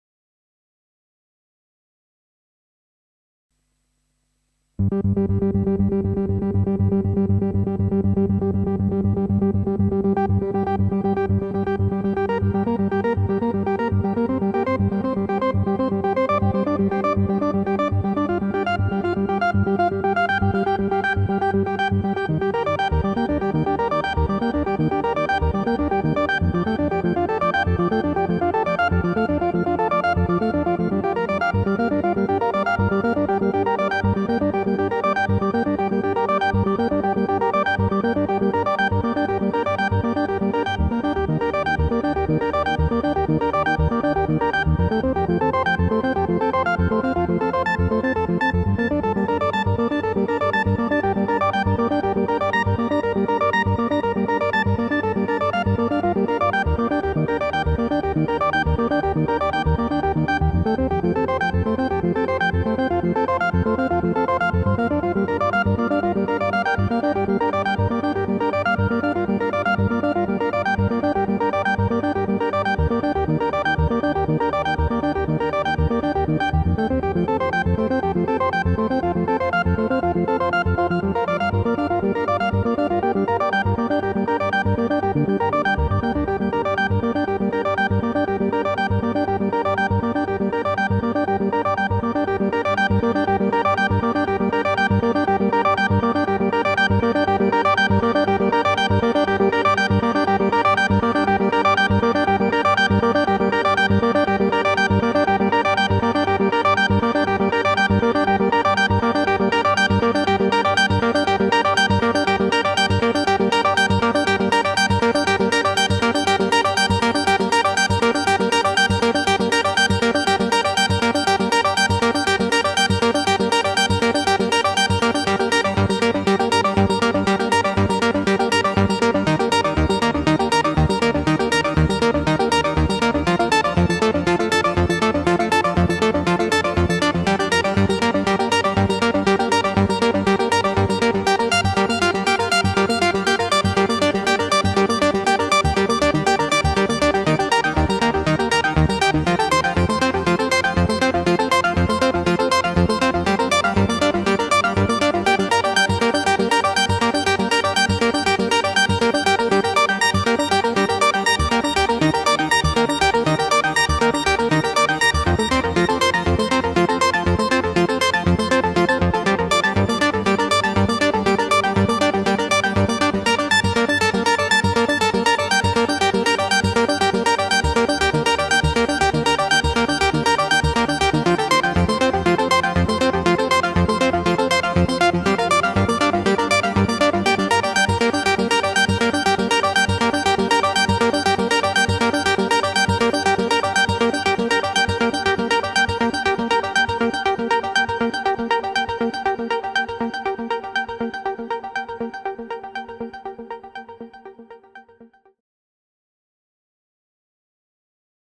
Korg Minilogue XD Arpeggio Improvisation Part1
Arpeggio Improvisation with a Korg Minilogue XD using the built in oscillators the filter and the LFO running through the built in effect section. Long duration, perfect to jam along. The video was recorded with a DJI Osmo Pocket 3 for Video and a Zoom H2N for Audio. Editing was done in Logic Pro X iPad. Video editing was done with Final Cut Pro for iPad on an iPad Pro M4.